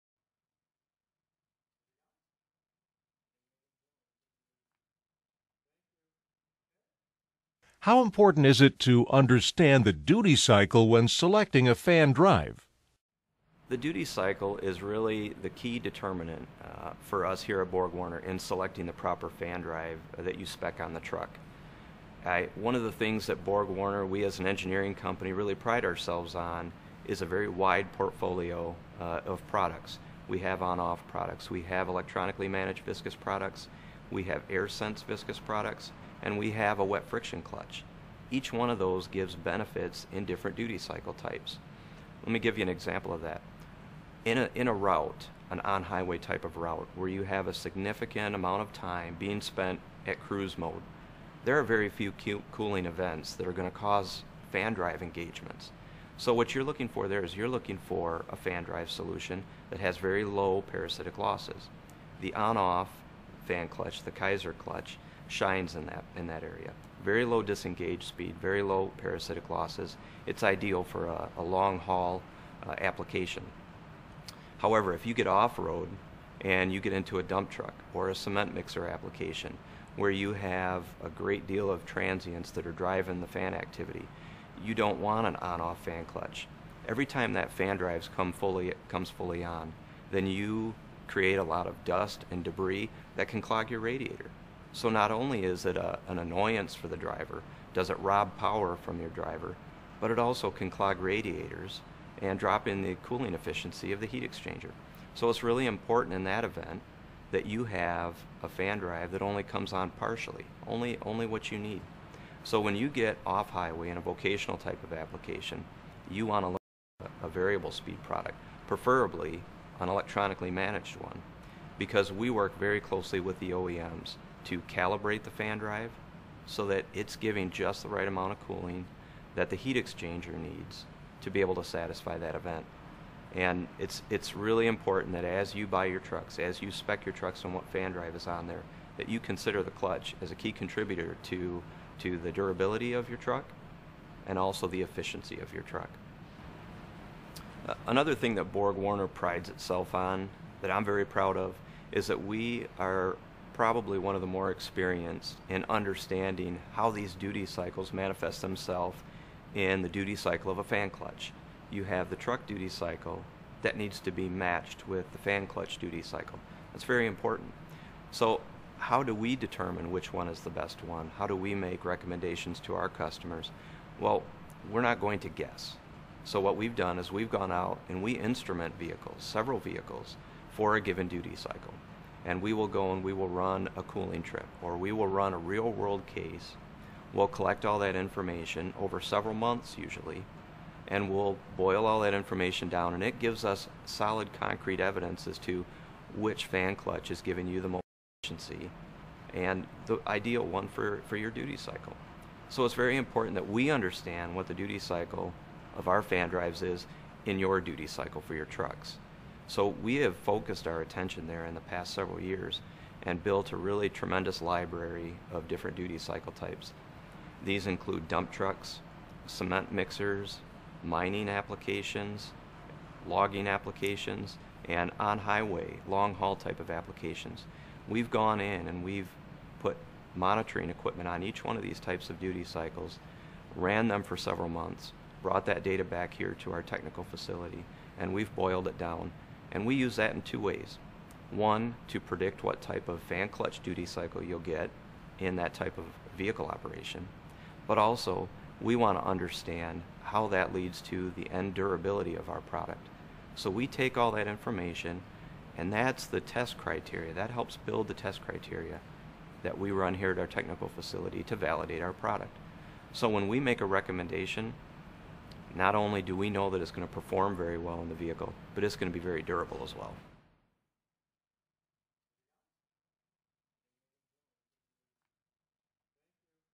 Borg Warner Benson Duty Cycle Interview
Second video in a four-part series, Borg Warner's Benson discusses the duty cycle.